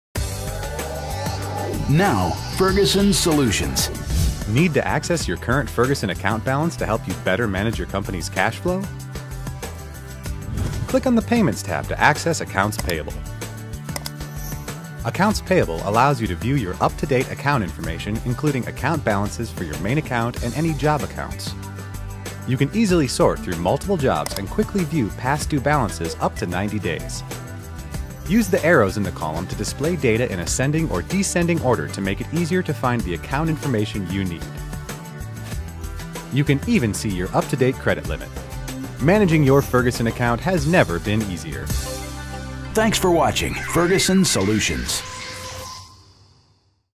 Ferguson Online: Accounts Payable
If you are in need of assistance when it comes to managing your company's cash flow, it can easily be accessed through Ferguson Online. Simply click on the "Payments" tab in the top right and select the "Accounts Payable" option. There, you can view up to date account information up to 90 days.